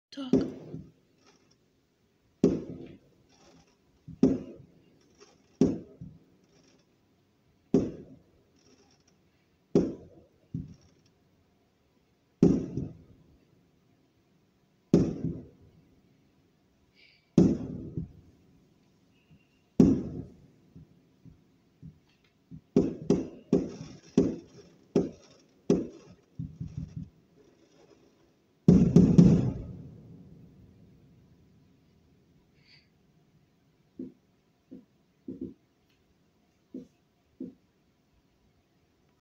4th of July Fireworks
Beautiful Fireworks for early 4th July 2023.